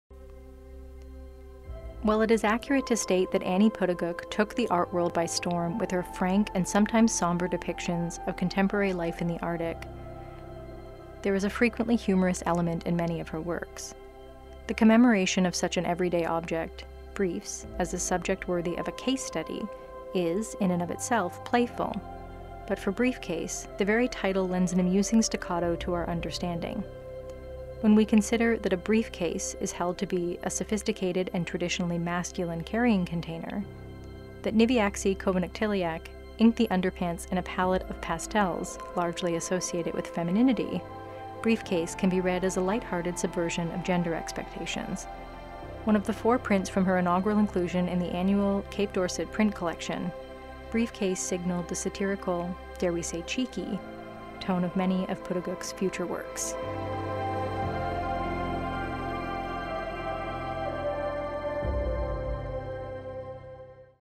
Lot 121, ANNIE POOTOOGOOK, Brief Case (Briefcase), 2005 #1
Lot 121
ANNIE POOTOOGOOK (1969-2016) KINNGAIT (CAPE DORSET)
Brief Case (Briefcase), 2005 #1
Printmaker: NIVIAKSIE QUVIANAQTULIAQ (1970-) KINNGAIT (CAPE DORSET)
lithograph on cream coloured paper, 17 x 17 in (43.2 x 43.2 cm)
12/50